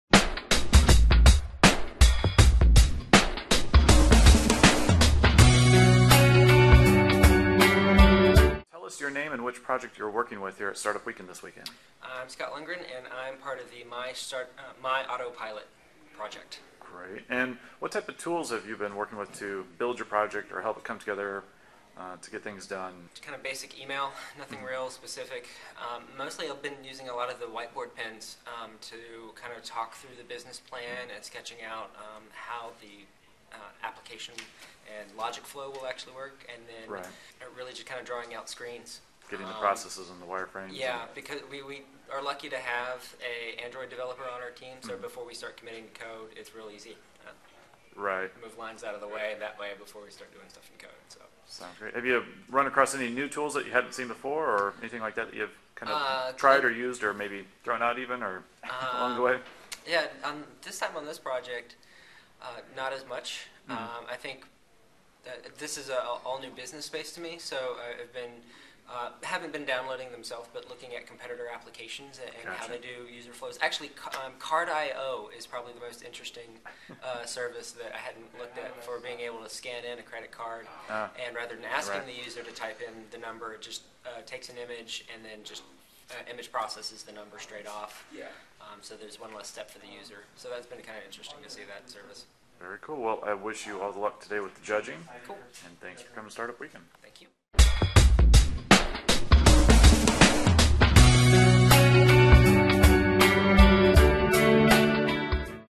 Scott Lundgren MyAutoPilot Charlotte Startup Weekend 2012 What Tools do you Use
Scott Lundgren from MyAutoPilot discussing tools used and discovered at Charlotte Startup Weekend 2012

For more information on Charlotte Startup Weekend, check us out

Video by Brett Bumeter of Softduit Media (one of the Organizers)